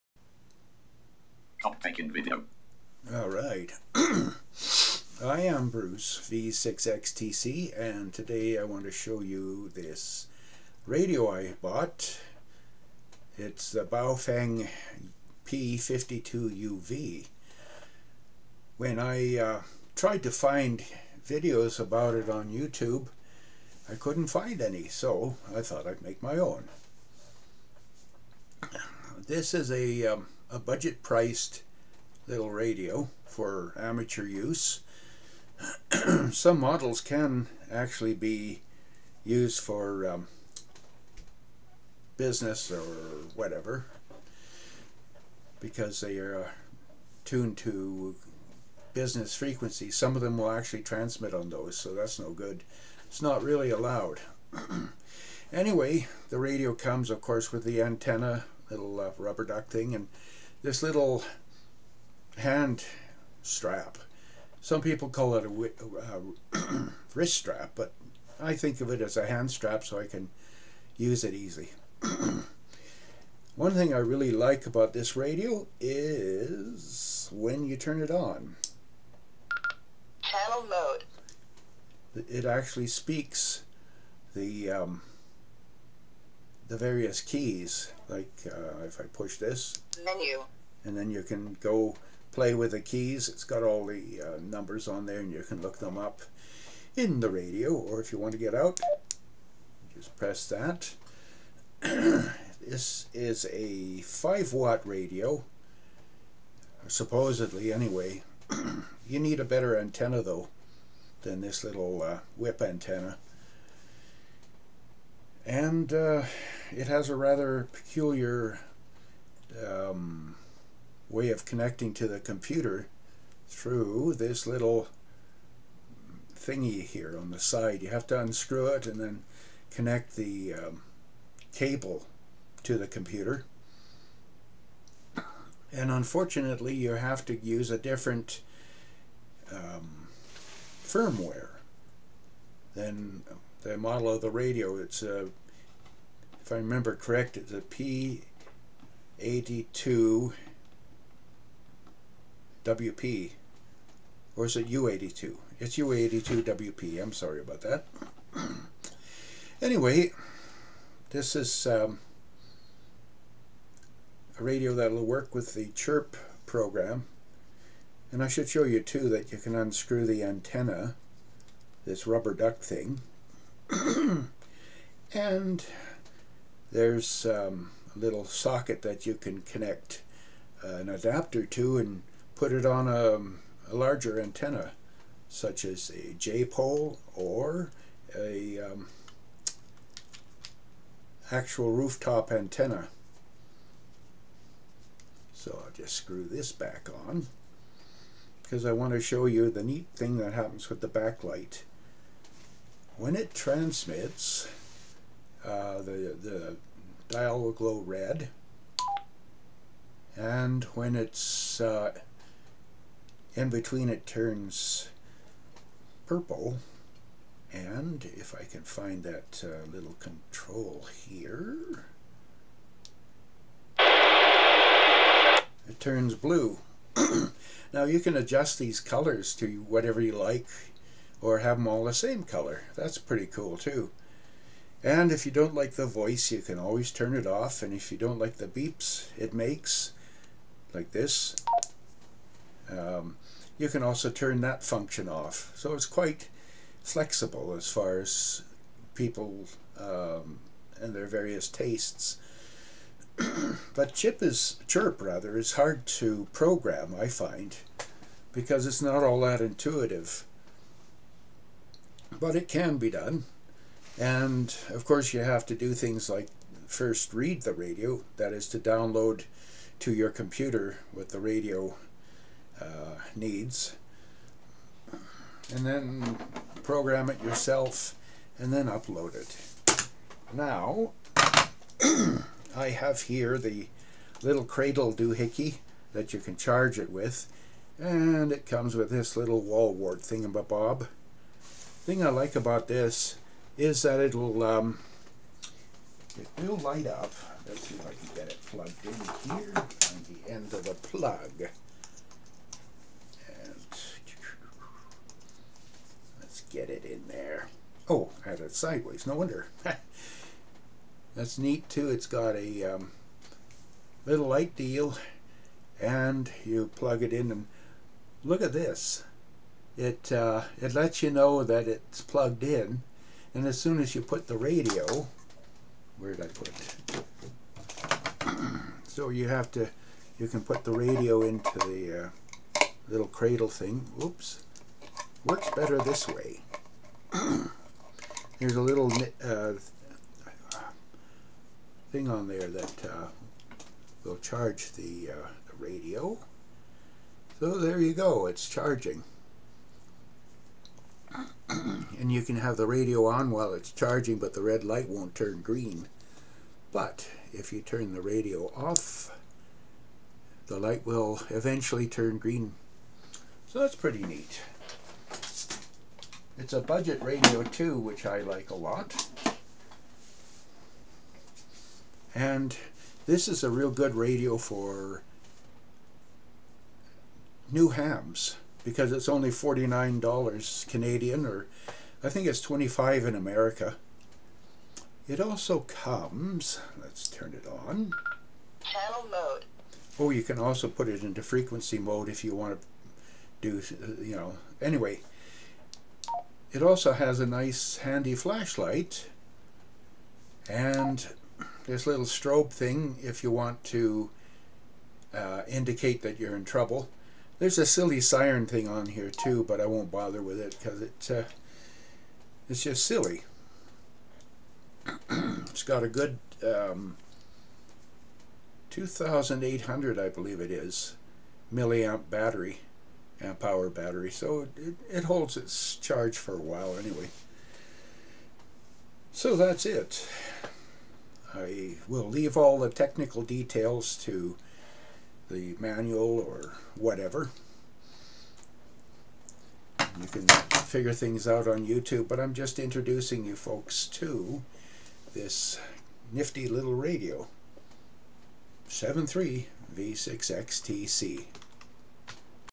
Baofeng P52UV quick review
Since nobody reviewed this little Chinese-made two-way transceiver for amateur radio use, I decided I'd make my own. It's just to introduce new hams to this beginner's radio. It's also mostly blind-friendly.